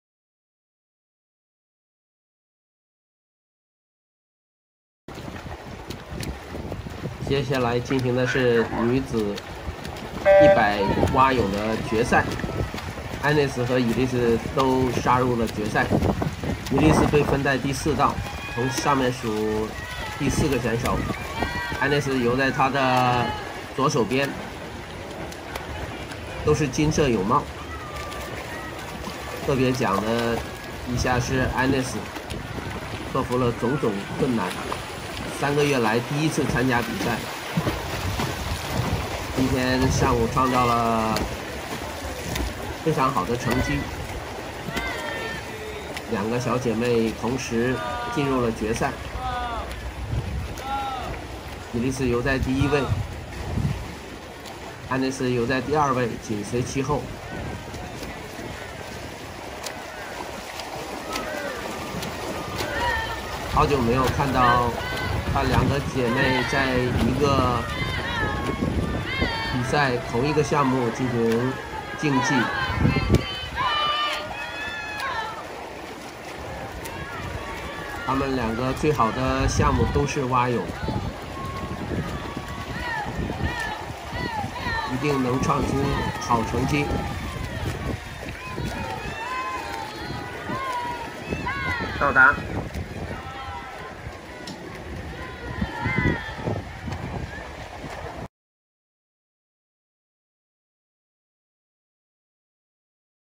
2021-07 BAC LC Swim Meet -100 Meter Breaststroke Final - Agnes and Elise
Date: 07/25/2021
Location: Burlingame Aquatic Club

100 Breaststroke Final
Elise : 1:23.98 (Personal best)
Agnes : 1:26.32 （Personal best）